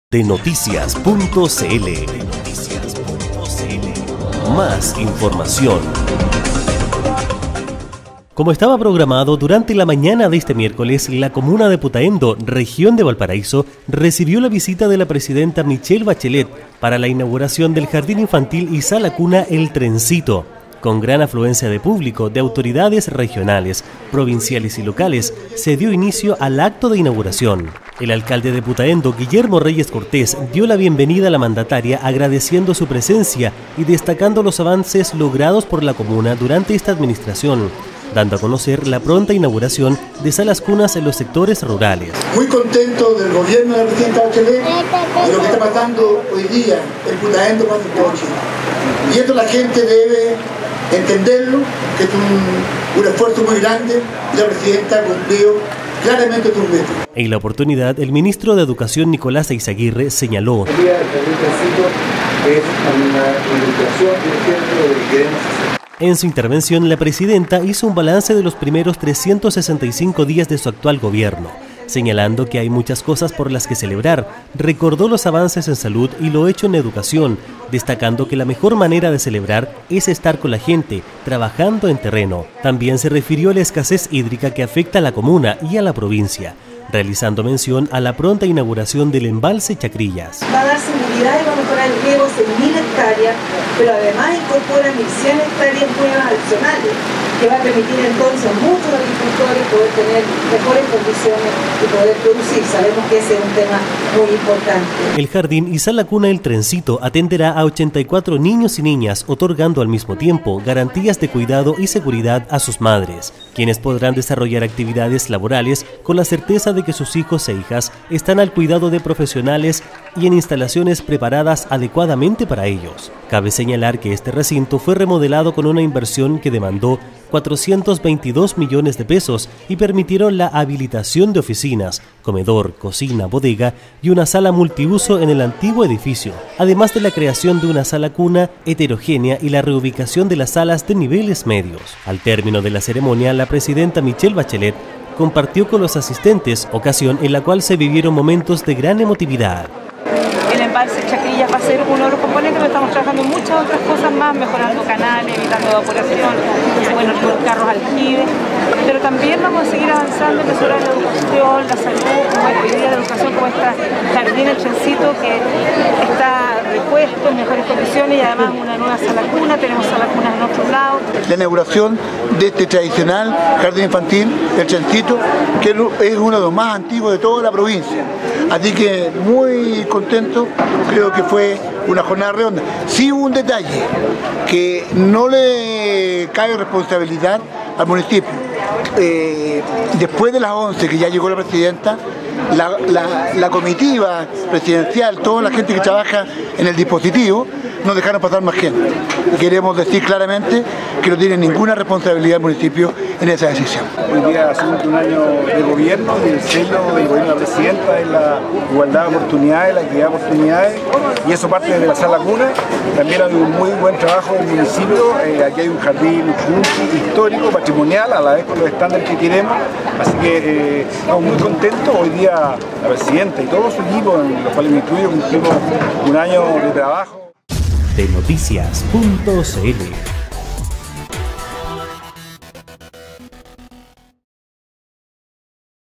INAUGURACION JARDIN INFANTIL Y SALA CUNA EL TRENCITO
visita de la presidenta michelle bachelet a la comuna de putaendo, en el marco de la inauguración del jardin infantil y sala cuna el trencito